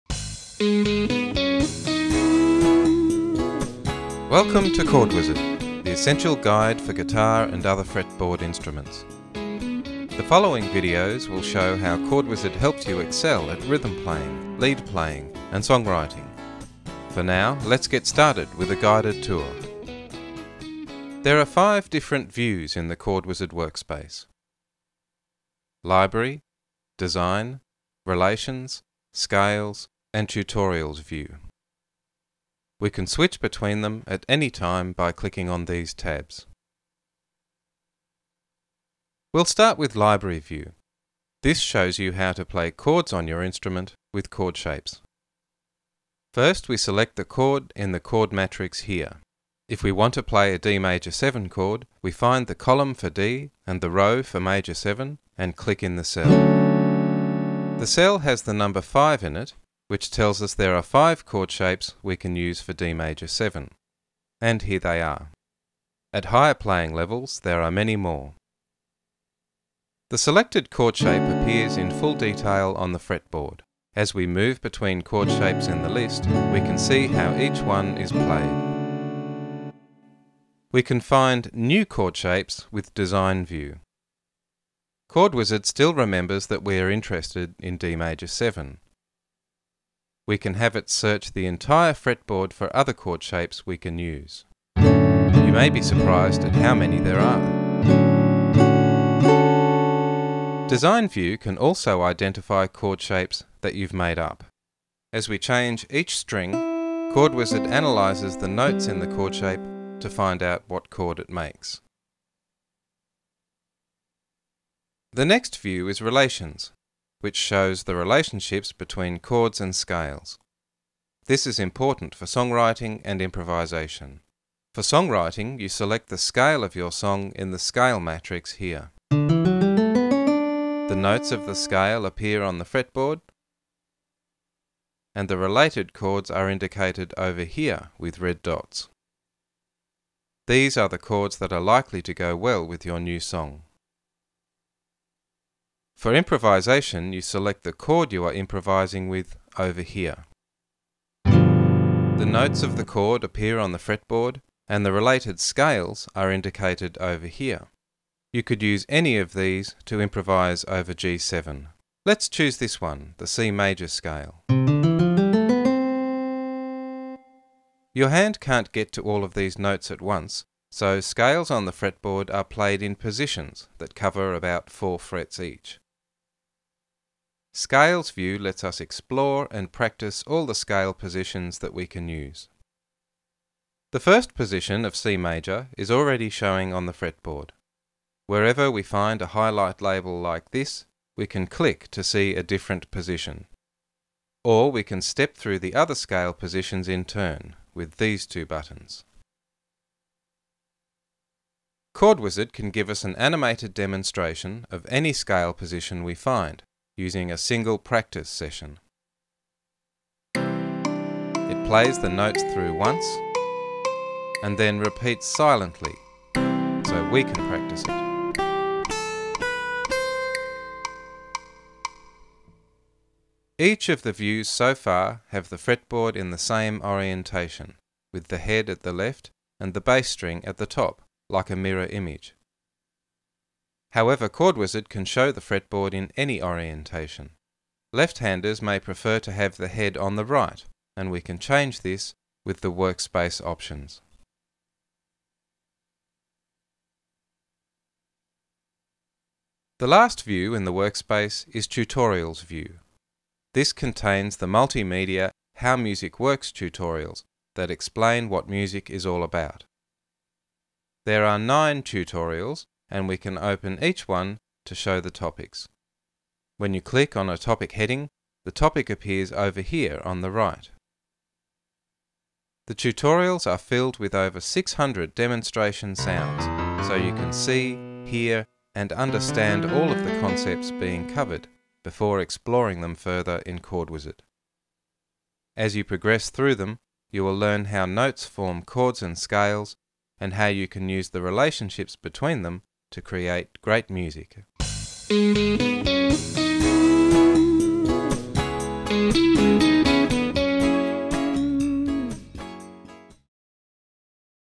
Video Tour for Overview of ChordWizard
A guided tour of the different areas of the ChordWizard workspace and how each area is used.